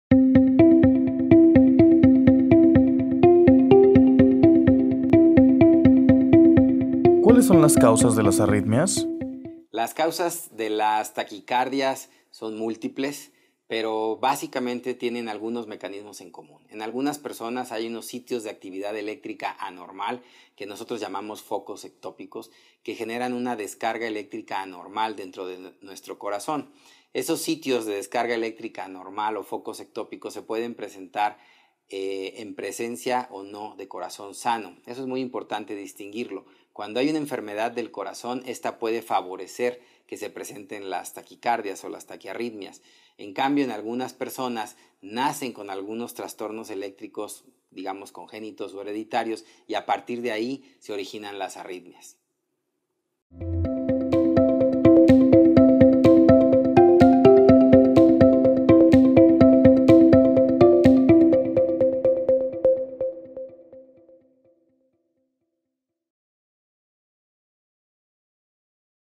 Enfermedades más comunes del corazón – Causas de las arritmias cardíacas
Una de las principales causas de arritmias cardíacas está relacionada con alteraciones en la regulación del ritmo por el sistema nervioso autónomo. Conoce más, de este padecimiento a través del Dr. Márquez Murillo, cardiólogo especializado en Electrofisiología y Electrofisiología Terapéutica.